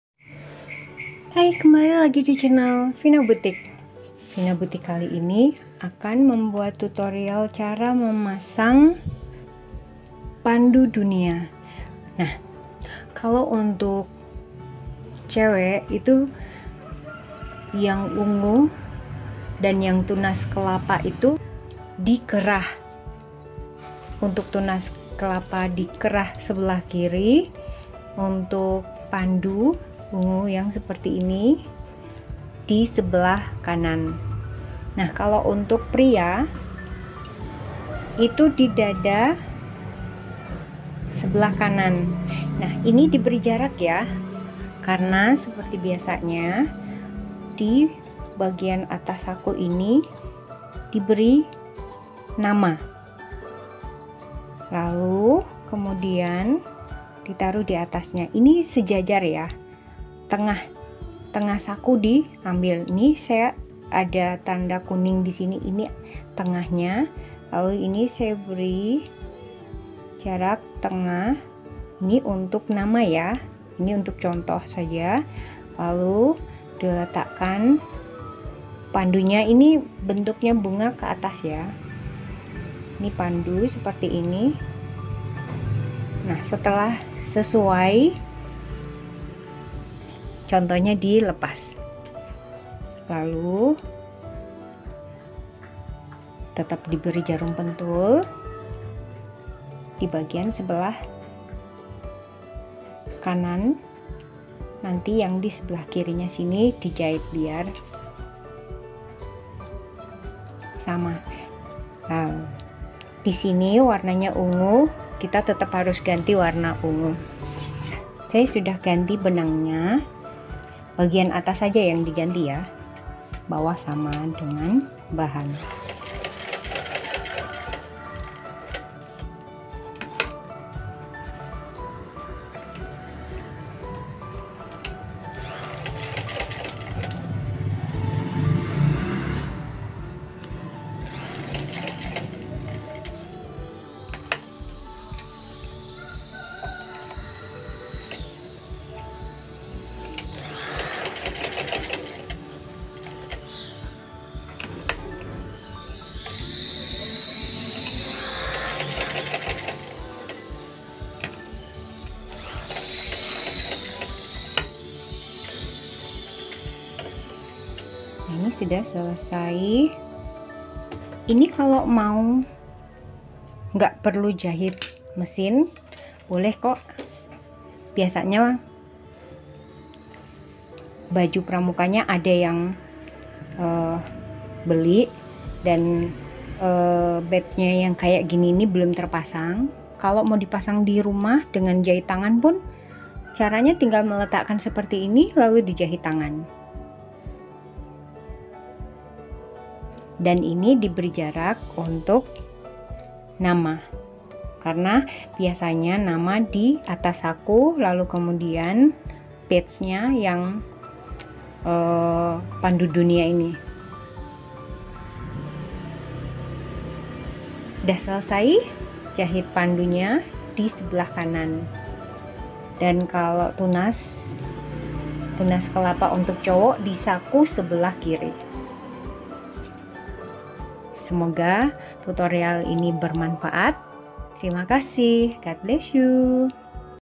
Tutorial Cara Pasang Badge Pandu Dunia di Seragam Pramuka Siswa Laki-Laki
Selamat datang di saluran YouTube kami! Dalam video tutorial ini, kami akan memandu Anda langkah demi langkah untuk memasang badge Pandu Dunia di seragam pramuka siswa laki-laki. Badge Pandu Dunia adalah salah satu lambang kehormatan dan prestasi yang sangat dihargai dalam gerakan pramuka.

Apakah Anda seorang anggota pramuka yang ingin memperoleh atau mengenakan badge Pandu Dunia dengan bangga? Atau mungkin Anda adalah seorang pemimpin pramuka yang ingin membantu siswa Anda dalam memasang badge ini dengan benar? Tak perlu khawatir! Di video ini, kami akan menyajikan instruksi yang mudah diikuti dan tips berguna untuk memastikan badge Pandu Dunia terpasang dengan rapi dan sesuai protokol.

Dengan penjelasan yang jelas dan demonstrasi yang baik, Anda akan memahami teknik-teknik yang tepat untuk meletakkan badge Pandu Dunia dengan presisi di tempatnya yang sebenarnya di seragam pramuka. Kami juga akan membahas tentang makna dari badge ini dan bagaimana kehadirannya dapat menjadi simbol semangat dan tanggung jawab dalam perjalanan pramuka.

Tonton video ini sampai selesai dan pastikan Anda tidak ketinggalan informasi penting apa pun. Jadikan badge Pandu Dunia di seragam Anda sebagai suatu kehormatan dan kesempatan untuk belajar lebih banyak tentang prinsip-prinsip pramuka yang akan membentuk Anda menjadi pribadi yang berdaya saing dan berbudi luhur.

Kami berharap tutorial ini bermanfaat bagi semua anggota pramuka, terutama siswa laki-laki yang bersemangat mengenakan badge Pandu Dunia dengan bangga!